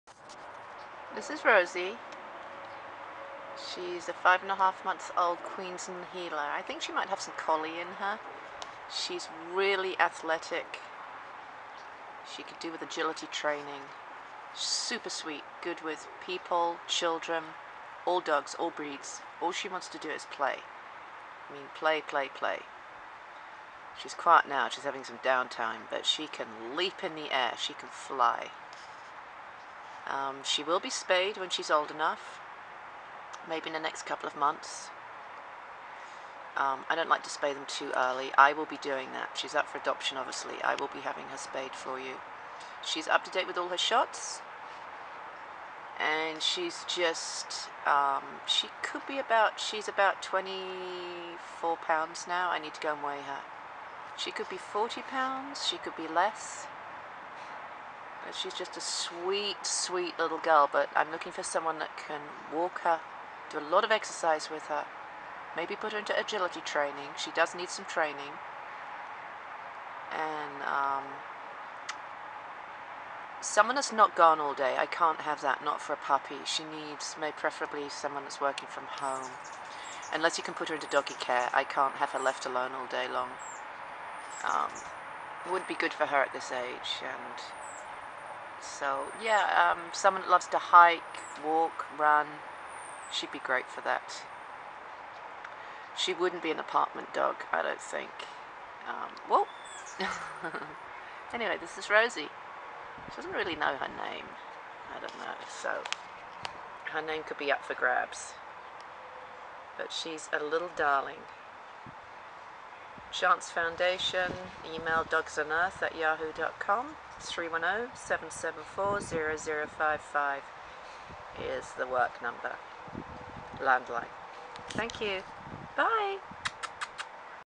Rosie puppy 5.5 month Queensland Heeler/Collie x
Rosie puppy chewing her toy, she's super sweet, great with people, children, other dogs all ages and sizes. She's very energetic this breed is, and can leap and jump very athletic, be a great puppy to put in agility training.  She weighs around 22lbs, in the video I said 40lbs that is what she could grow to be, hard to tell at this age what exact weight will be. She is up to date with all her shots and microchipped. I will have her spayed in a couple of months I don't like to have them spayed too early.